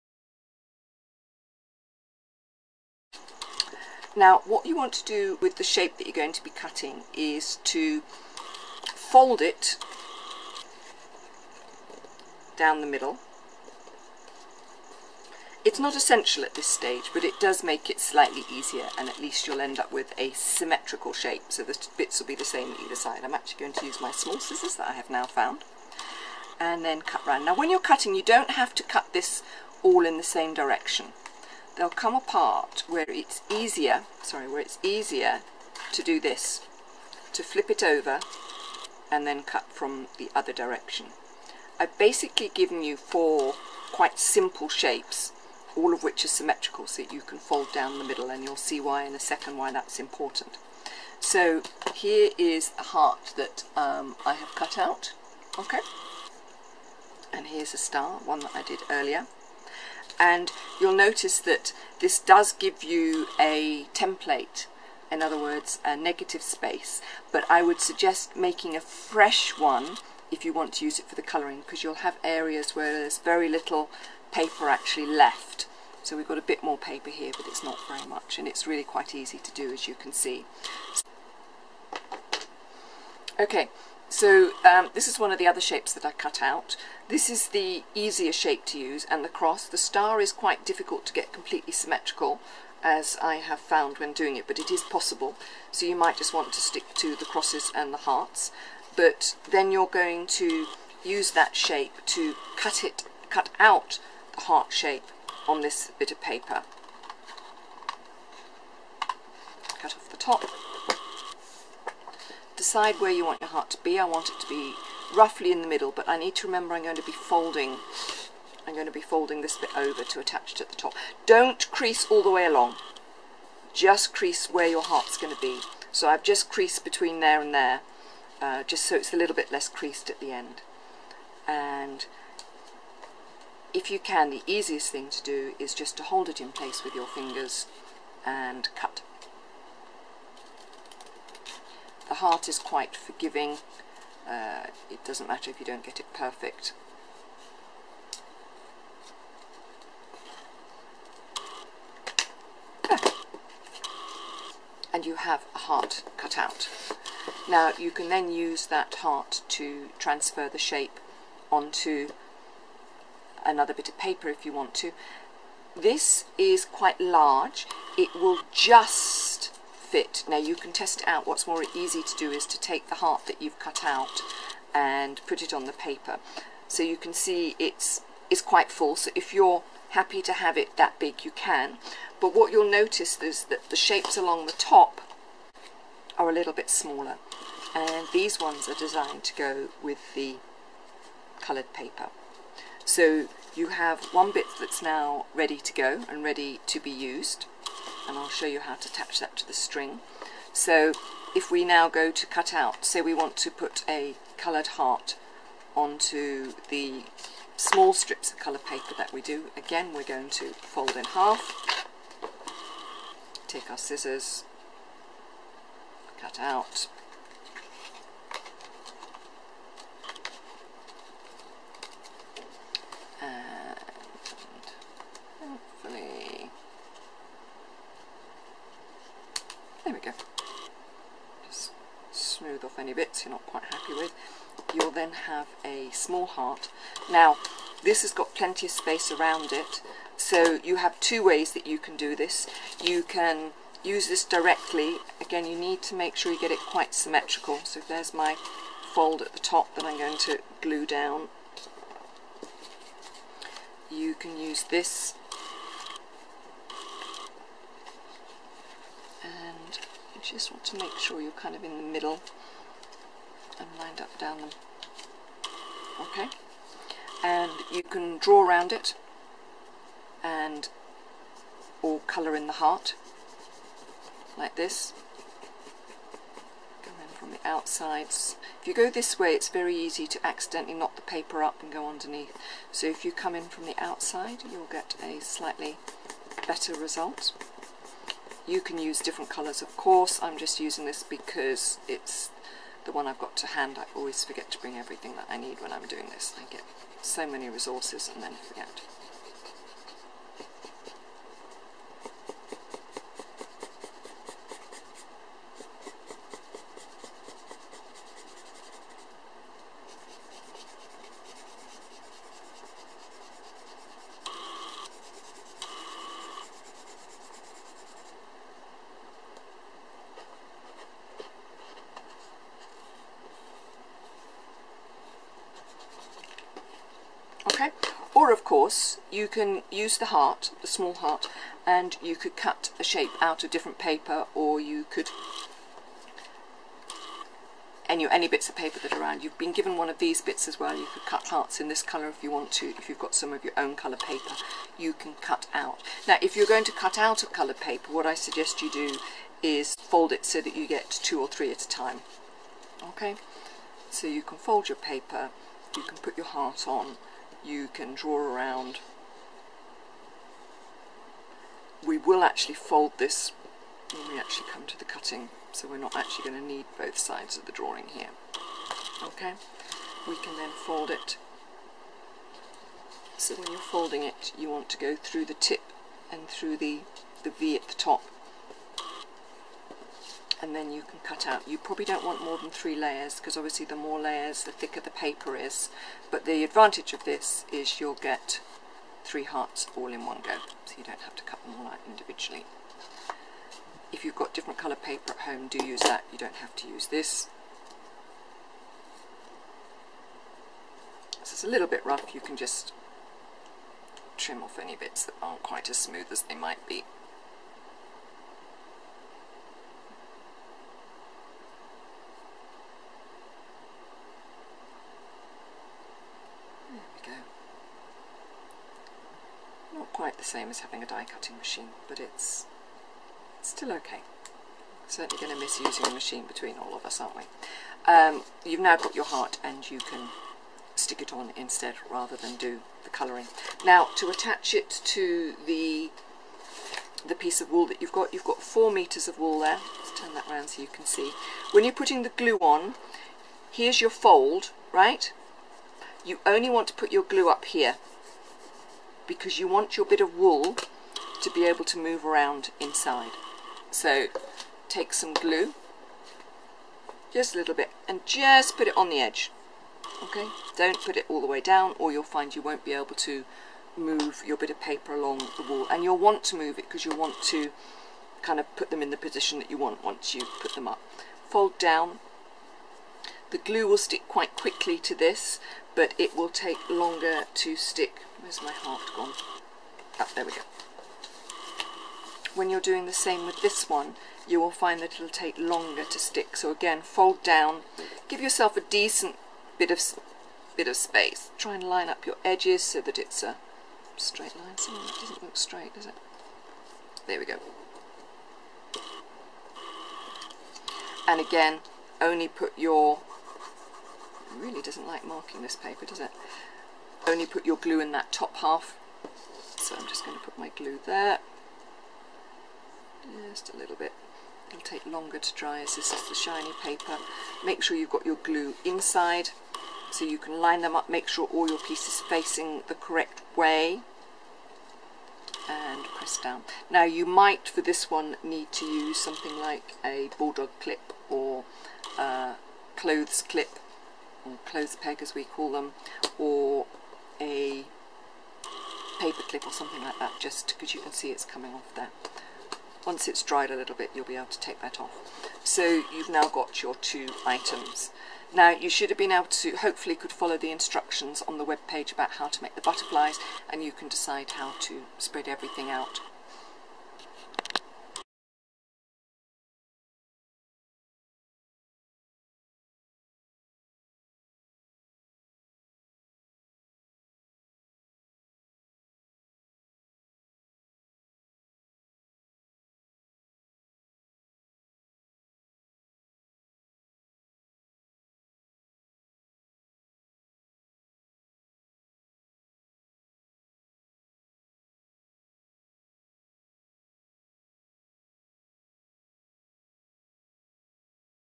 Cutting shapes out using printed shapes to get a stencil you can use.
For cmc Bagayam craft clubs but these instructions may be useful for others.
How to cut out the shapes and which papers to use for which sized shapes.
for links to all the different parts of the garland.